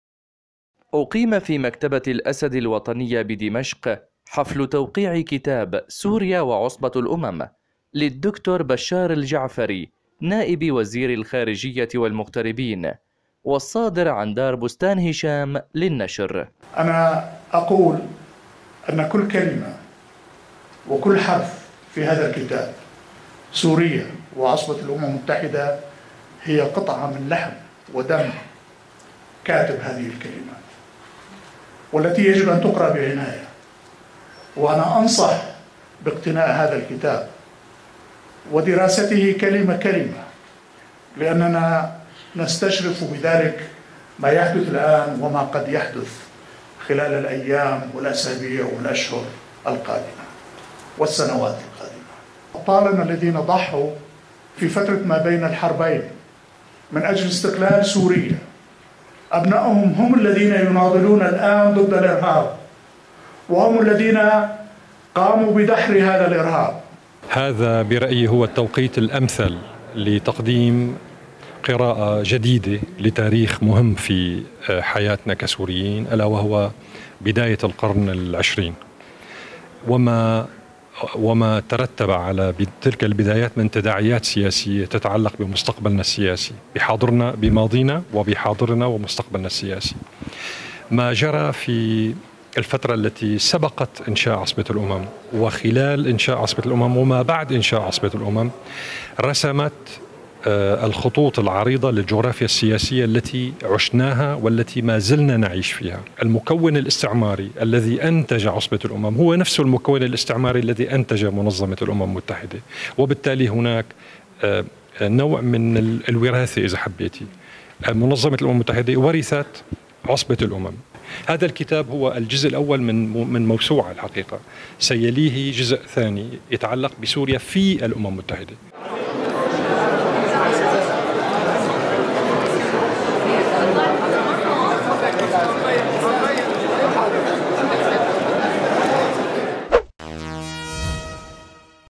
Book launch by Dr Bashar al Jaafari: Syria and the League of Nations
The publication of “Syria and the League of Nations” by Dr. Bashar al Jaafari, Deputy Minister of Foreign Affairs and Expatriates, was celebrated at a packed house ceremony in the Al Assad National Library, on 12 July. The Damascene auditorium was filled with Syrian ministers, including Minister of Culture, Dr. Lubana Mushawah, members of the diplomatic corps, journalists, intellectuals, and people interested in their country’s significant history.